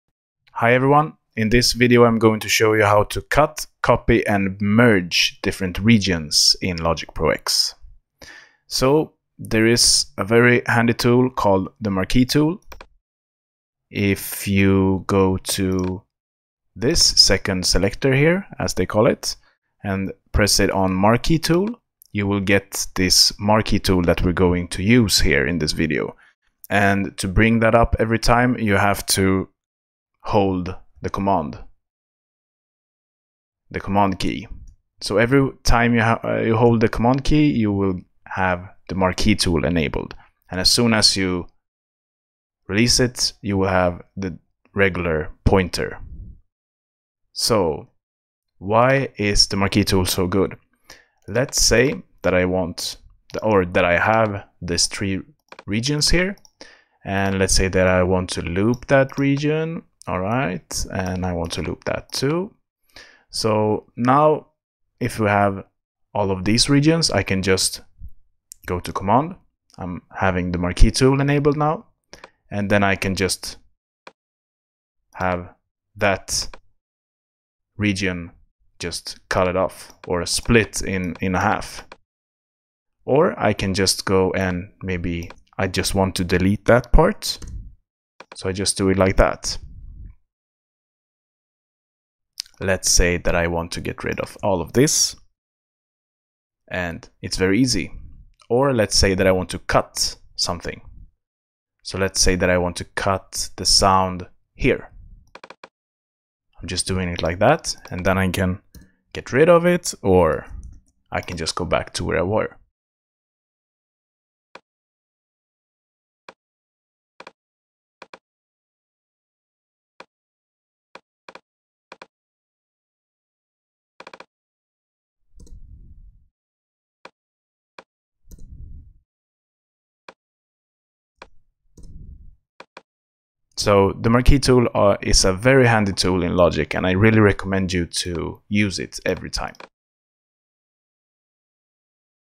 Cut, Copy & Merge in Logic Pro X
Learn different workflow tips in Logic Pro.